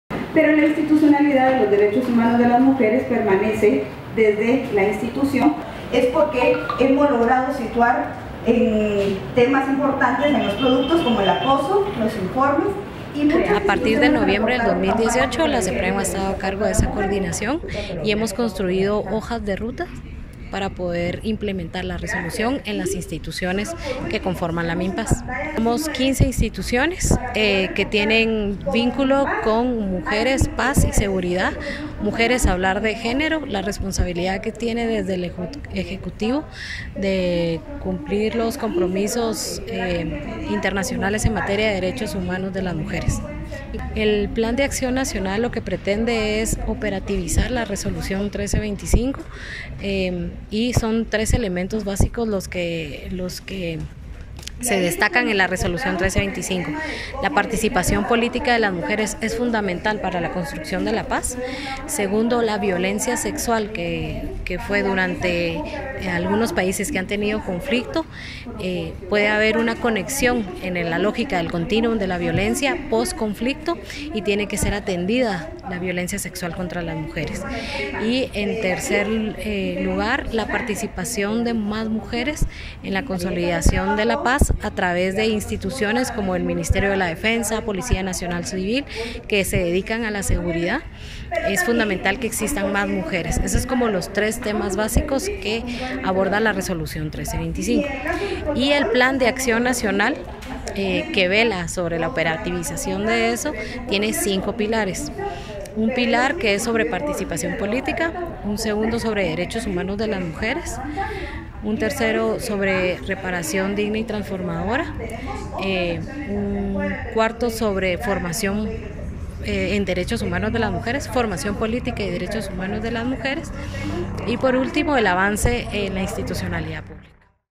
Fortalecen acciones en favor de las mujeres
Para continuar fortaleciendo las acciones en favor de las mujeres, la Secretaría Presidencial de la Mujer (Seprem) llevó a cabo la reunión de trabajo de la Mesa Interinstitucional sobre Mujeres, Paz y Seguridad (Mimpaz), con el objeto de elaborar la hoja de ruta 2020.